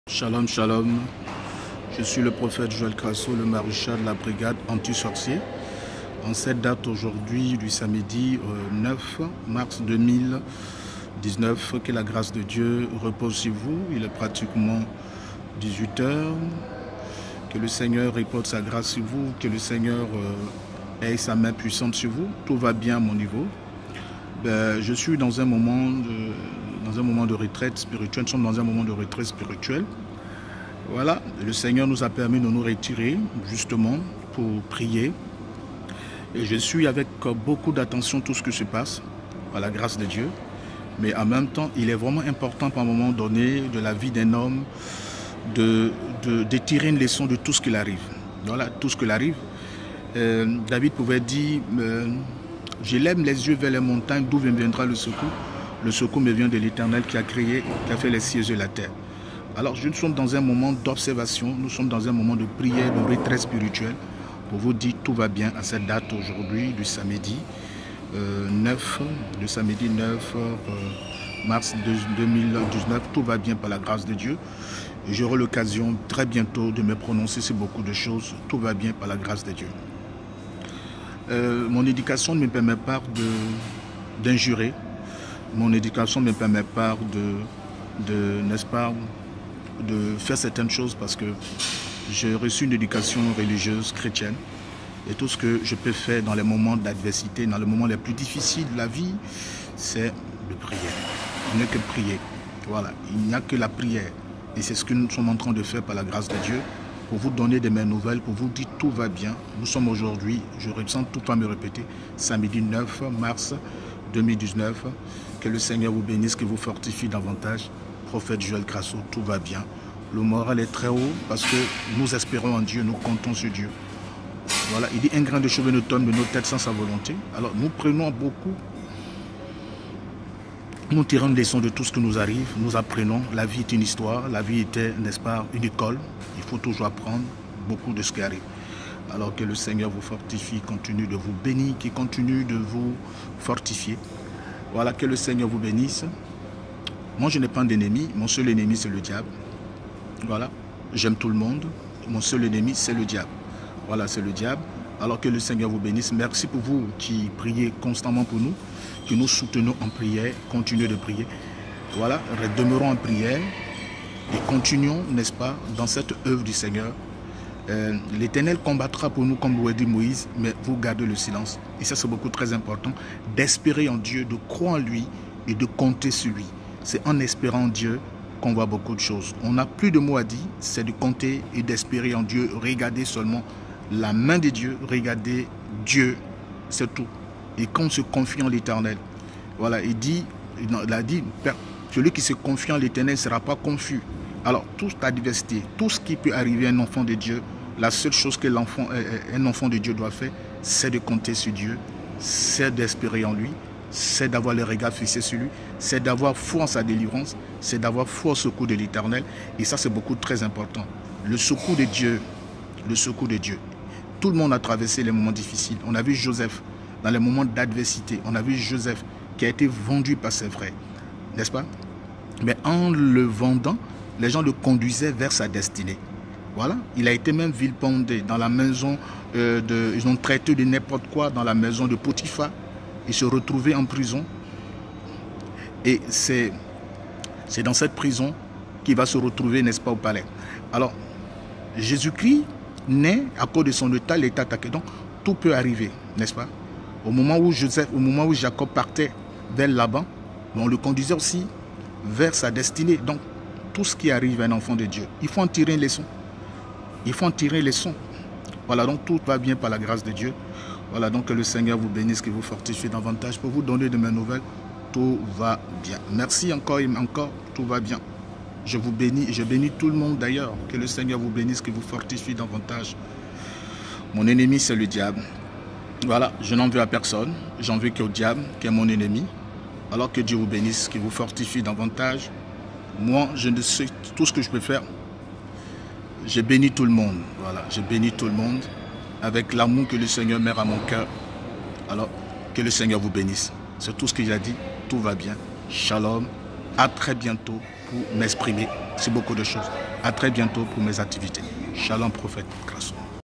SCANDALEUX !!! Prophète Joël Krasso sort du silence et dénonce un complot
Enfin toute les vérités mises à nu